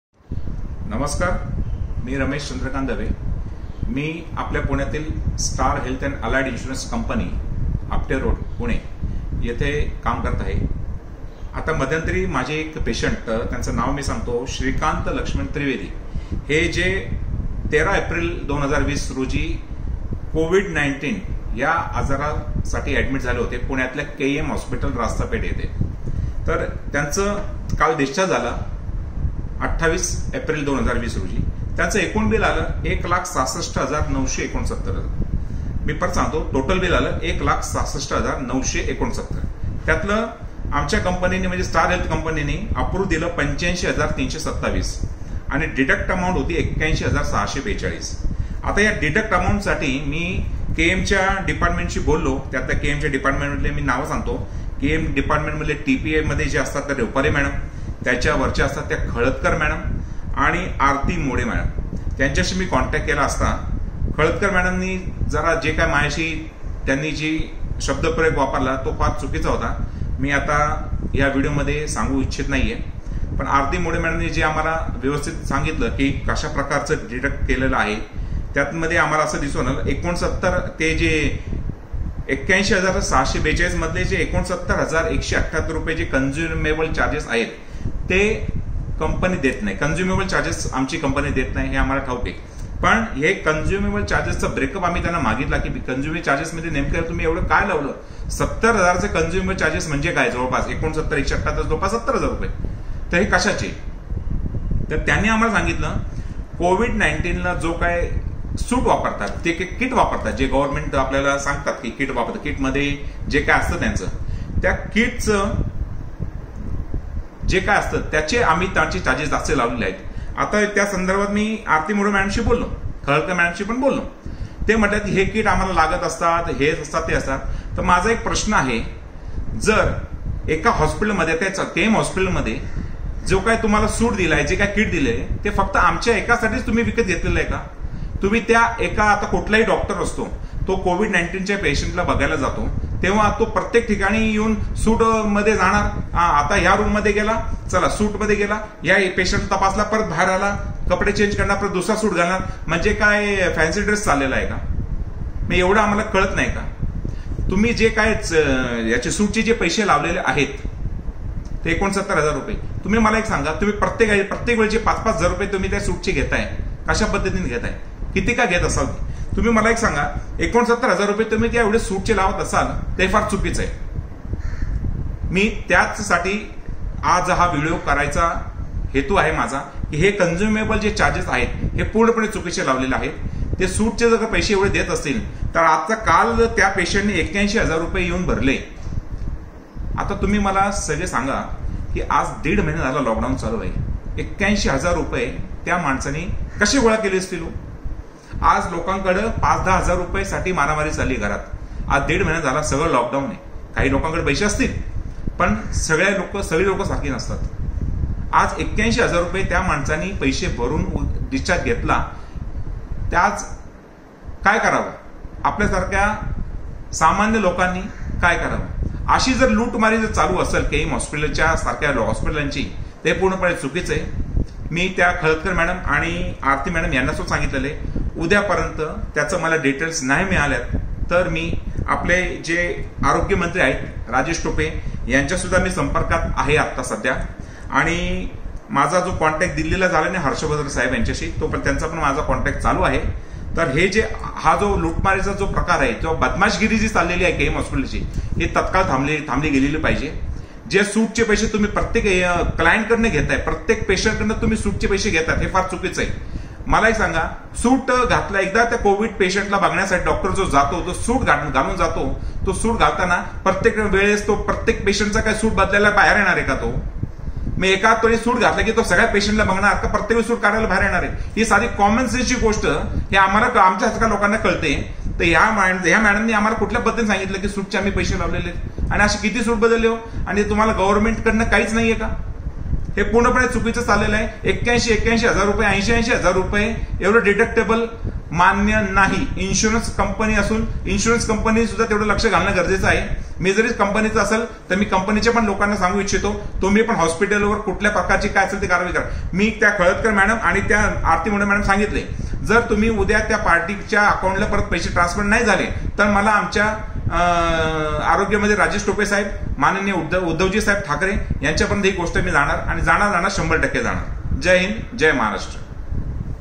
SBT24TV MARATHI : पुण्यातील के ई एम हाॅस्पिटल येथे कोरोना रुग्णांची लुट*
पुणे येथील के ई एम हाॅस्पिटल येथे कोरोना रुग्णांची खुले आम लुट चालू आहे,
एकादी व्यक्ती कोरोना बाधीत आहे, तिला सोडताना
 हाॅस्पीटलने पैसे मागितले ती वक्ती गरीब आहे,ती व्यक्ती पैसे कोठून आणणार व पैसे कसे भरणार काहि लोकांचे खाण्याचे हाल आहेत,
बरेच कोरोना बाधीत रुग्ण मध्यम वर्गीय आहेत,
आशी लोक के ई एम हाॅस्पिटल सारख्या दवाखान्यात पैसे कसे भरणार
*शासनाने यावर लक्ष केंद्रित करावे*
अशी अनेक हाॅस्पिटल आसु शकतात ॽॽ